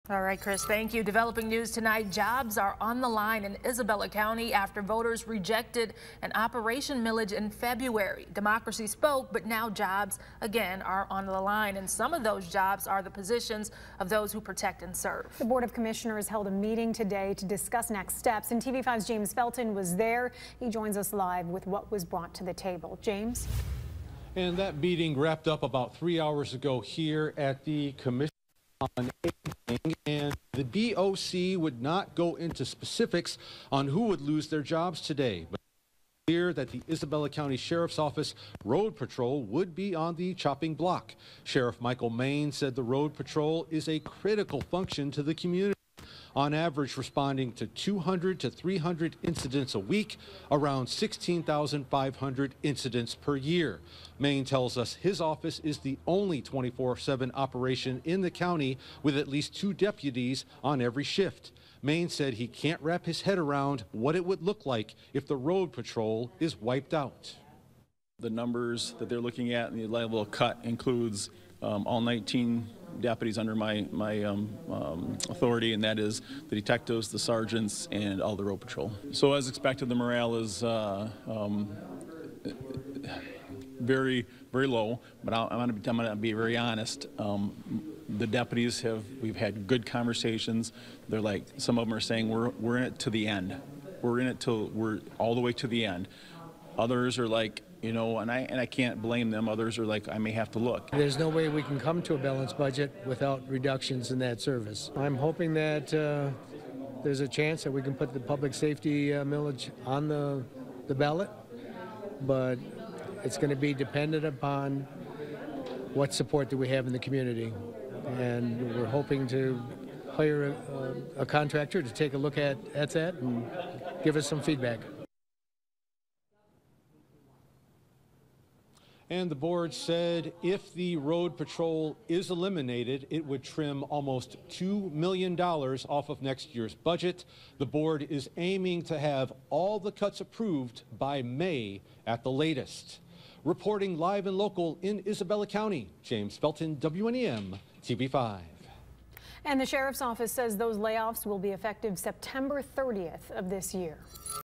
Cuts coming to Isabella Co. road patrol
Among job cuts being made in Isabella County, the Board of Commissioners made it clear that the sheriff's office road patrol would be on the chopping block.